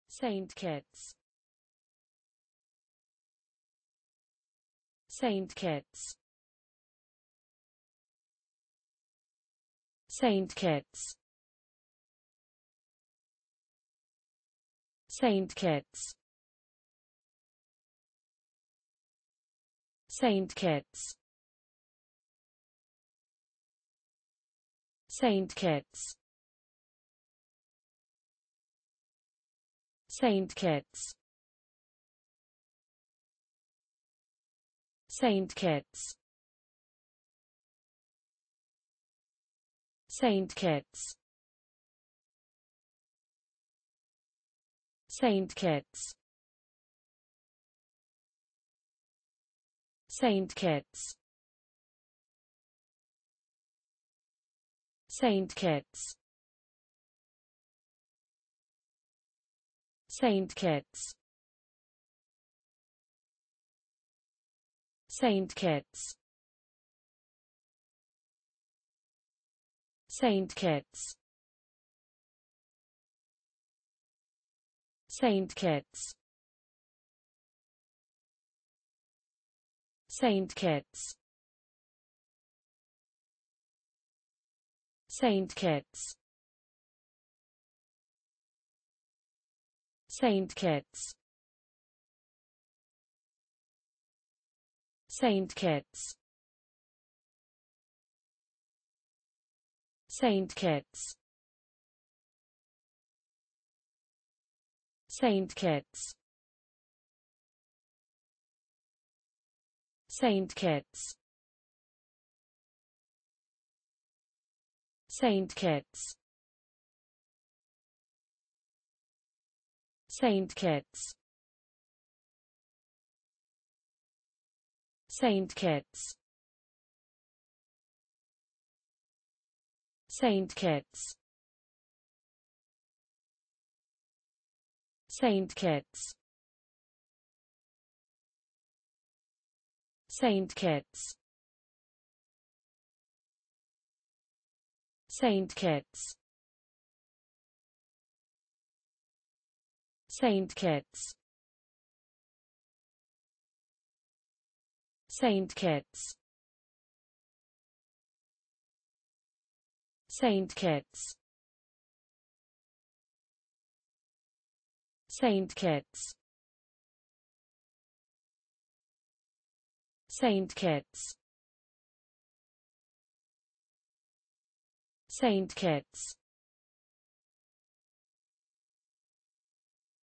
Saint Kitts. How to pronounce the english word Saint Kitts .Start with S. Learn from me.
Follow me!Learn from me how to pronounce the english word Saint Kitts.This video shows you how to pronounce the English word Saint Kitts .